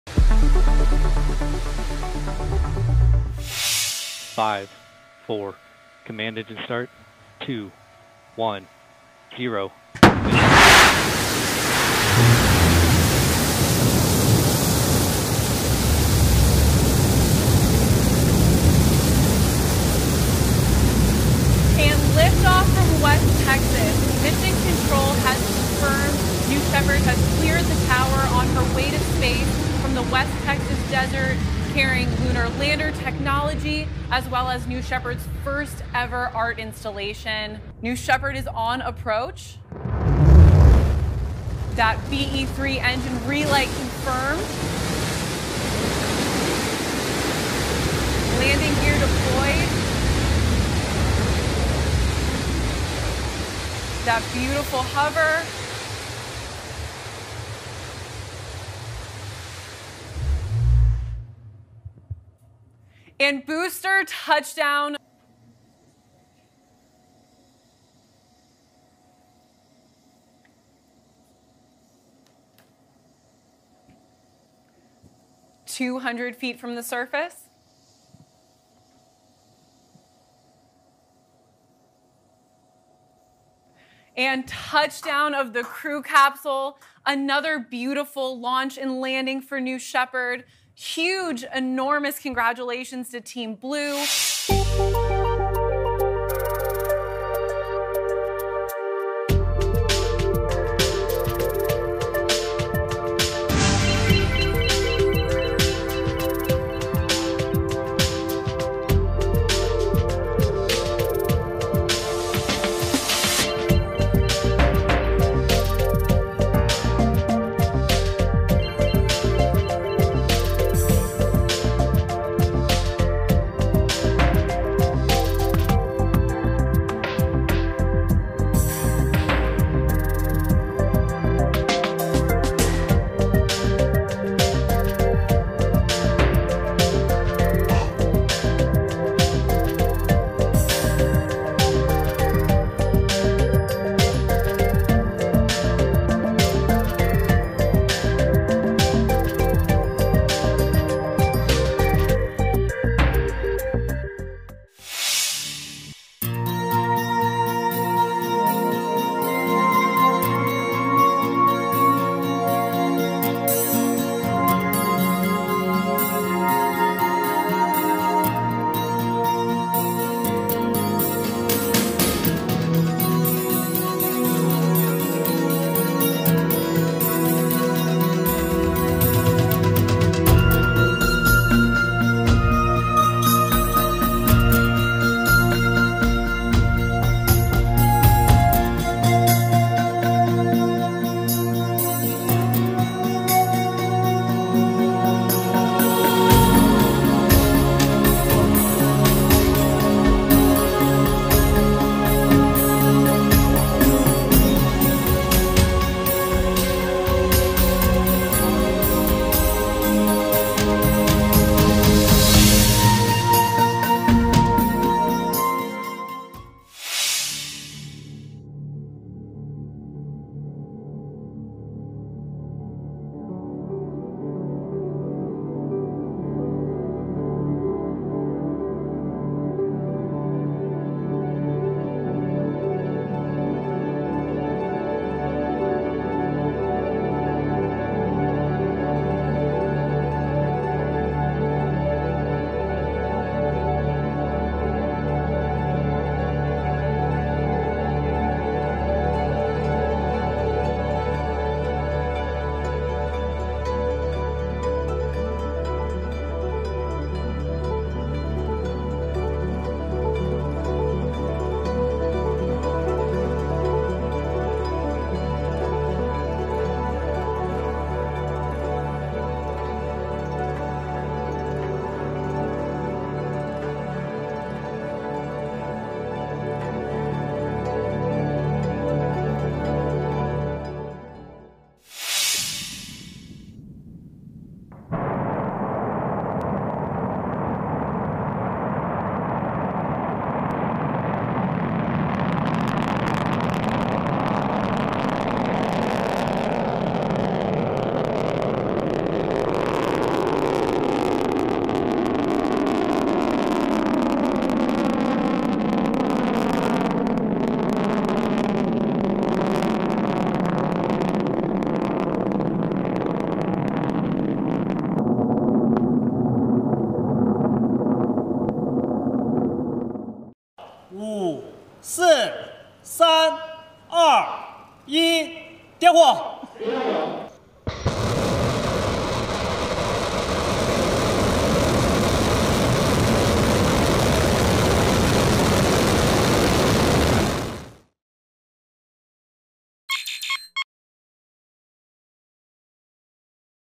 Blue Origin launch highlights, rover sees Martian moon and more in VFS Weekly
Blue Origin launched the NS-17 mission, Perseverance saw the Martian moon Deimos, Hurricane Grace and Tropical storm Henri were seen from space, see a timelapse of Ingenuity's 12th flight on Mars, and China launched a pair of rockets on the same day.

Blue Origin launches New Shepard to test moon landing tech and more in suborbital flight